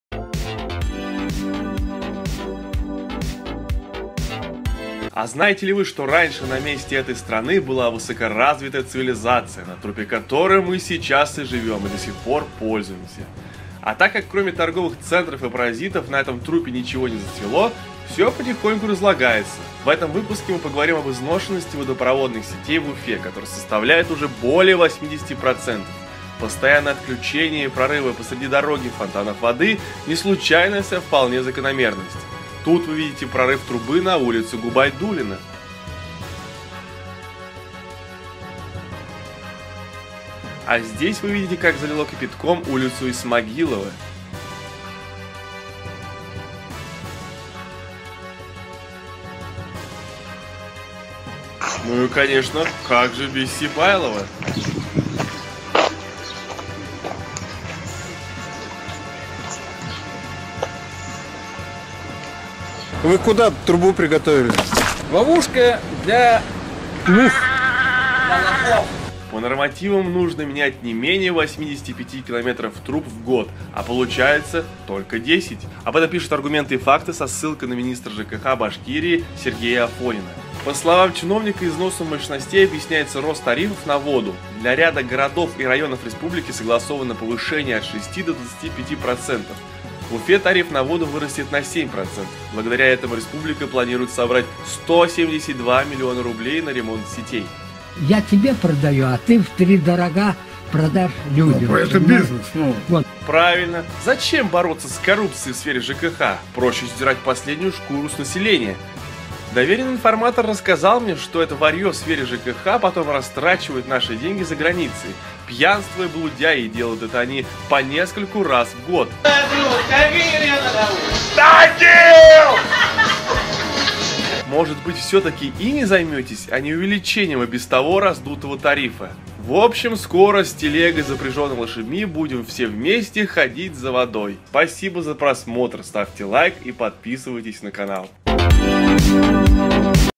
80% водонапорных труб изношены (сюжет)
Прорвало трубу?Залило водой?Не переживай, это только начало.
как долго послужат эти трубы?и что нас ждет?